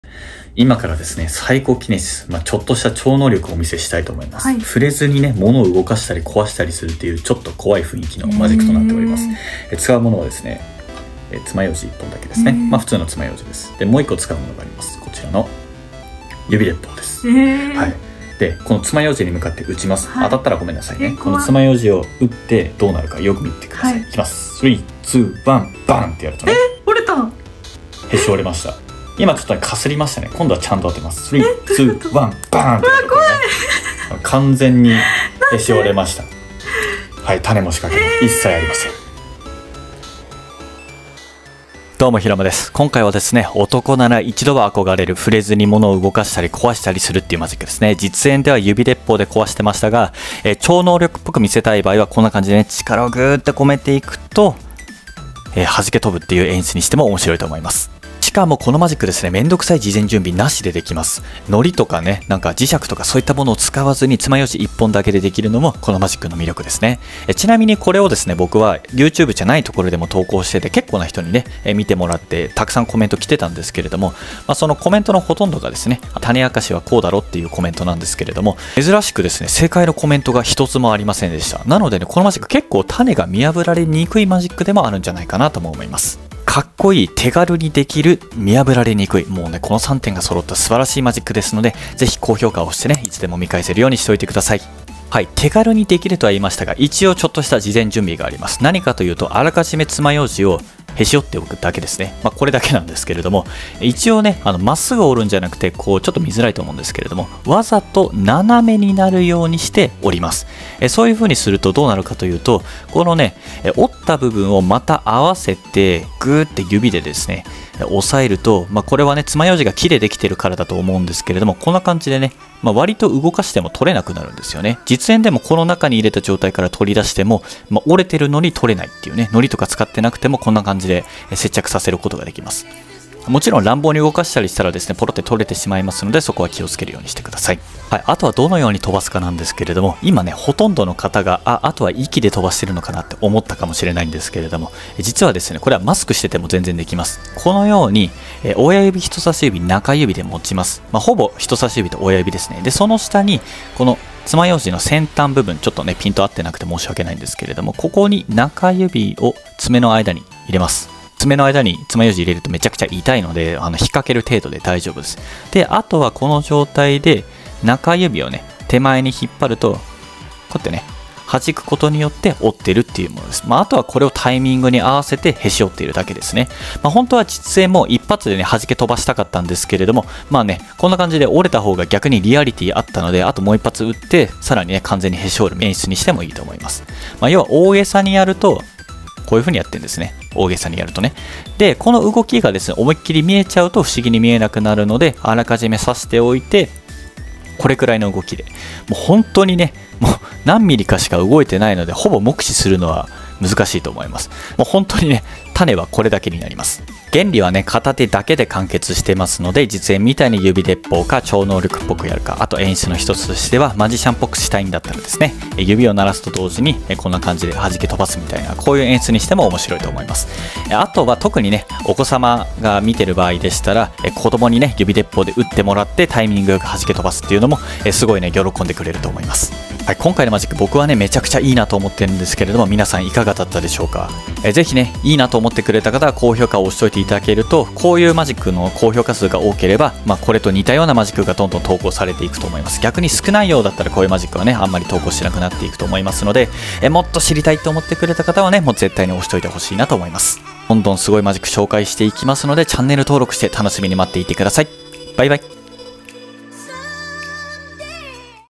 信じられないけどマジです。誰でも簡単、触れずに物をブッ壊す超能力マジックのタネ明かし
2024年もよろしくお願いします。

今回は超手軽にできる超能力っぽいマジックの解説です。
小ネタではありますが、盛り上がりやすいのでぜひ練習して習得してみてください。